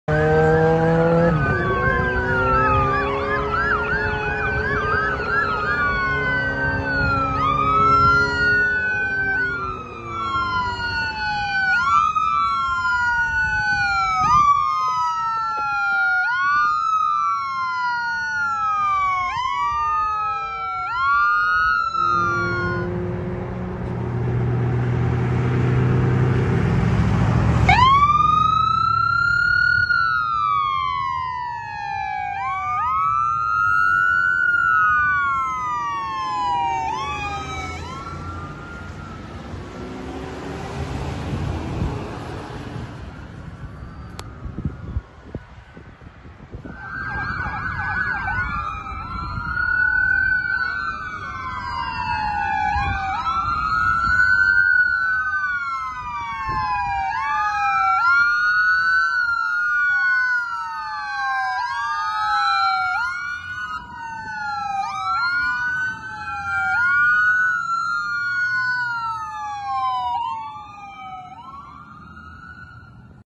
ALS RENDEZVOUS: Police Escort EMS To Hospital After Serious Incident in Lakeshore - Essex-WindsorEMS
Here’s video of the emergency run to hospital after an incident in Lakeshore a few days ago.

ALS Rendezvous = Advanced paramedics that help with critical patients on the way to hospital when the ambulance is still on the way to hospital

They discuss a location to meet at so they can jump out of one ambulance and into another as treatment is still going on

ALS = ( Advanced Life Support )